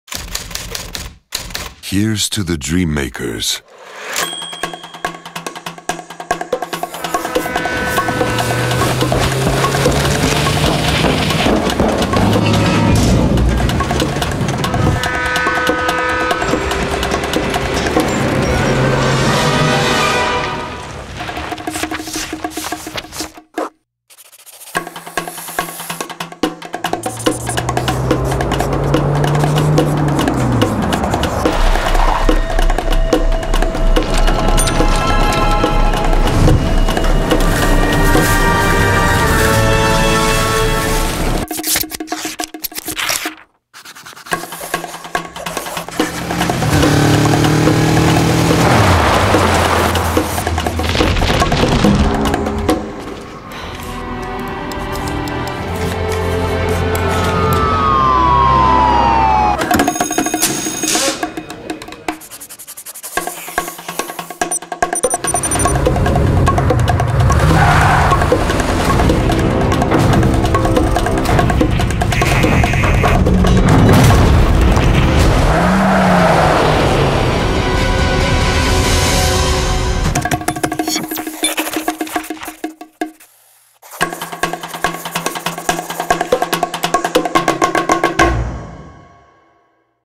Honda Dream Makers | VFX Breakdown
Honda dream makers features four characters, who each drive a different model of Honda in their respective stories. Each of them races toward the conclusion of a plot that is already in progress. Interestingly, various stages of the process behind the creation of each segment is shown in detail, while the action unfolds.

Post-Production Company: Time Based Arts
3D: Chris Wood, Dan Davie, Tom Robinson, Stephen Ross
Executive Producer: Tom Johnson
Colourist: Simone Gratarolla
Concept Artists: Sylvie Minois, James Husbands, Yibi Hu, Nigel Raynor, Ben Oliver
3D Artists: Federico Guzzardo, James Mann, Oscar Gonzalez Diez, Dave LohMike Battcock, Sam Osbourne, Federico Vanone, Nigel Timms, Jeroen Hooghoudt, Tom Hall, Jess Gorick
Nuke Artist: Ralph Briscoe, Aitor Arroyo, Linda Cieniawska
Nuke: Matt Shires, Bernie Varela (Lead)
Flame Artists: Jamie Crofts, Adam Paterson
Flame: Luke Todd, Matt Jackson, Thiago Dantas (Lead)
VFX Supervisor: Sheldon Gardner, Stephen Grasso

Connect with Artistmesh